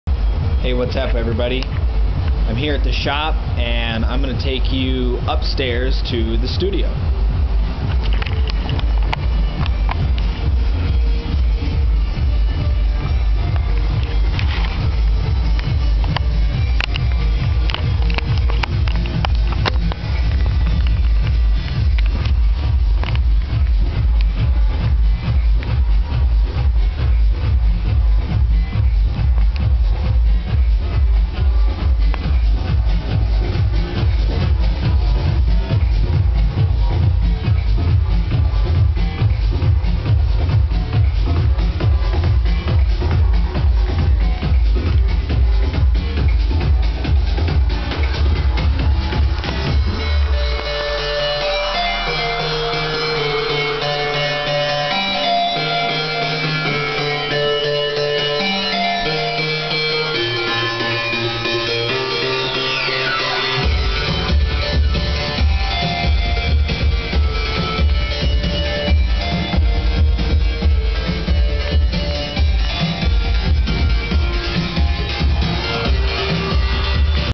Quick tour of my studio in Fargo, ND
My buddy is surprised when I come up with a camera!